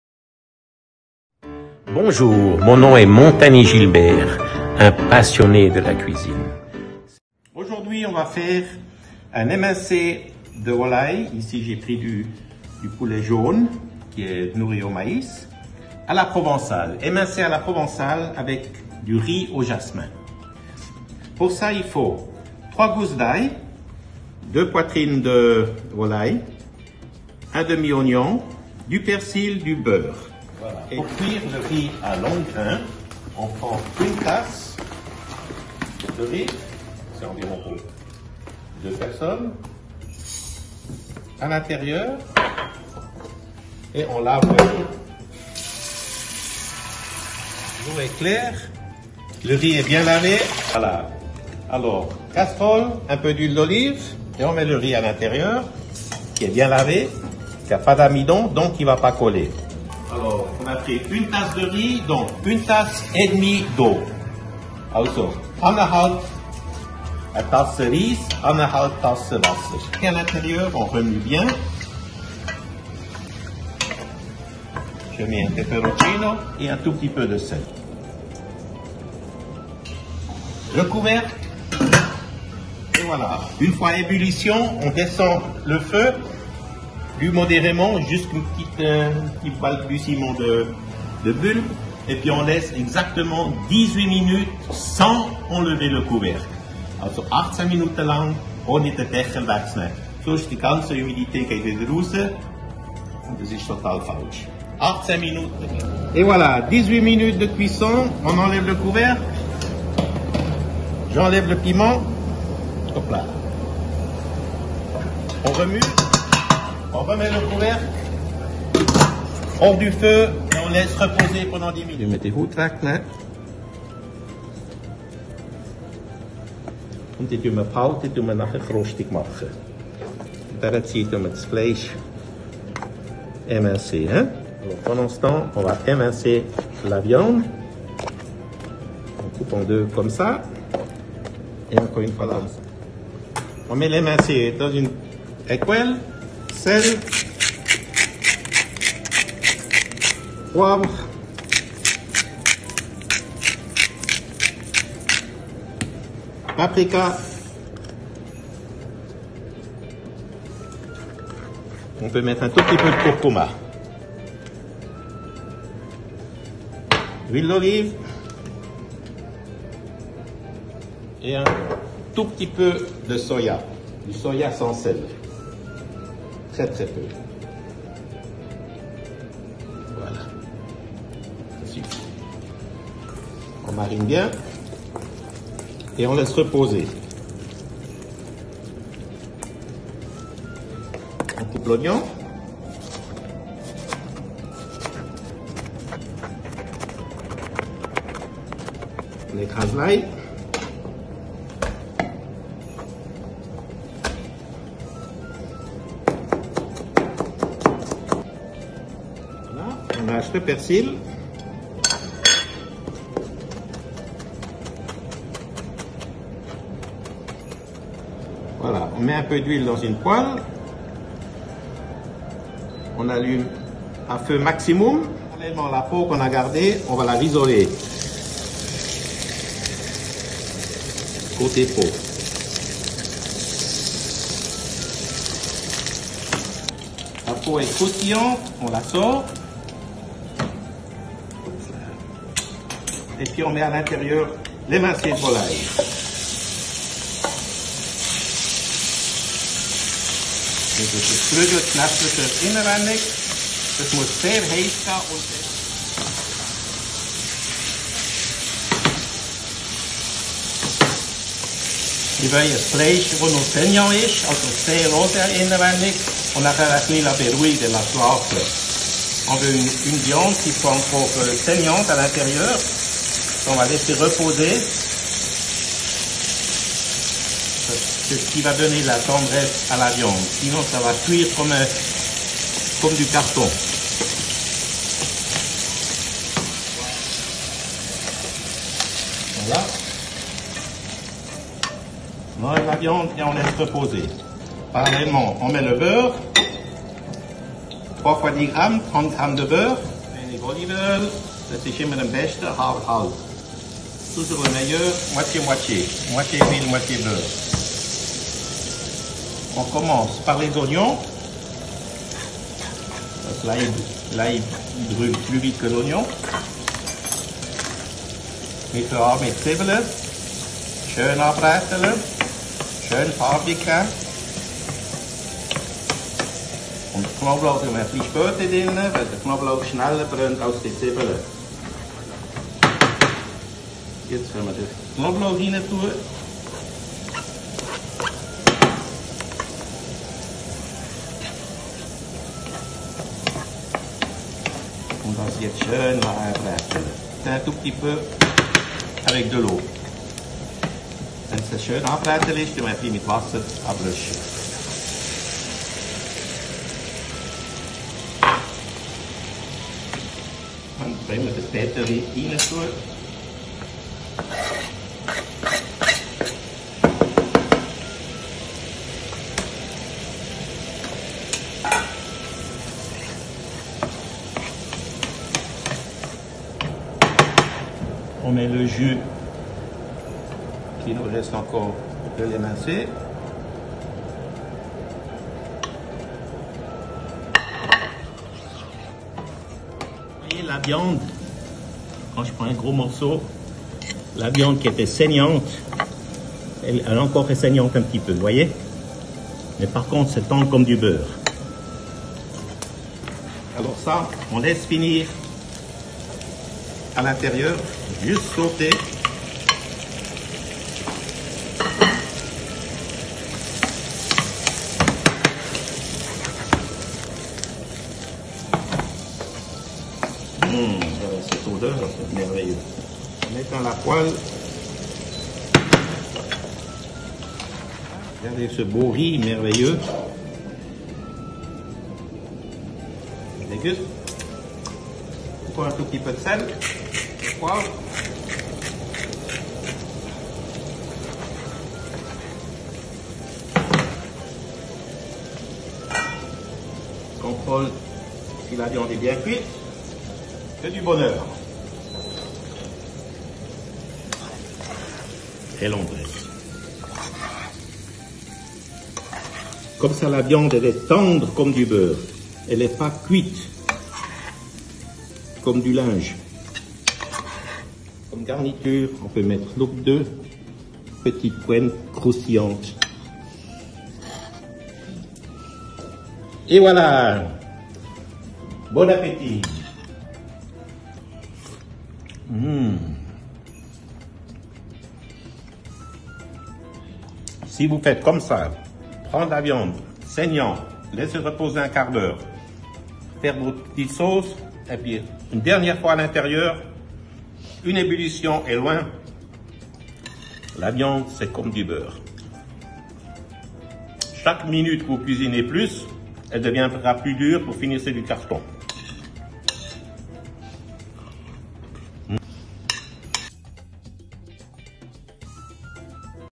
Émincé de volaille provençale
Recette Émincé de volaille provençale :
- prendre 2 poitrines de poulet jaune, émincé
- Hacher 1 demi oignons
- Hacher 3 gousses d’ail
- Hacher 1 bouquet de persil plat
- 30 gr. de beurre, huile d’olive
- 1 cac (cuil à café) de Soya
- Sel, poivre noir, paprika, curcuma

Cuisson :
- Rissoler l’émincé durant 1 min dans l’huile
   très chaude, sortir la viande saignante et
   laisser reposer 15 min.
- Mettre 30 gr. de beurre et l’huile d’olive dans
   la poelle  chaude (moitié-moitié)
- Rissoler l’oignon puis l’ail, sel, poivre,
- Ajouter 1/2 dl d’eau ou bouillon
- Réduire et ajouter le persil
- Contrôler l’assaisonnement
- Insérer la viande, donner une ébullition 1 min
- Servir avec du riz créole